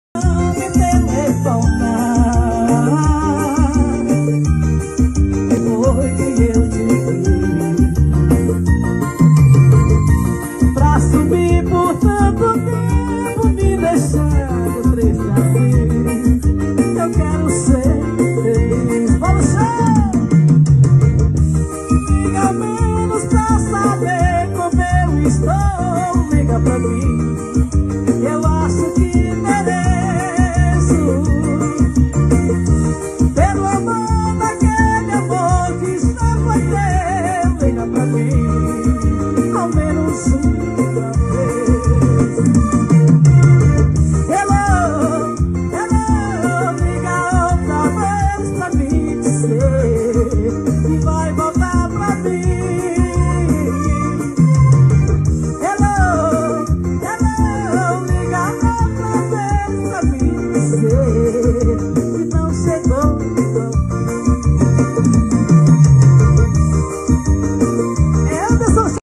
Aristela Vasconcelos morre em acidemte em Piripiri
A cantora sofreu um acidente na BR-22 na madrugada deste comingo (06)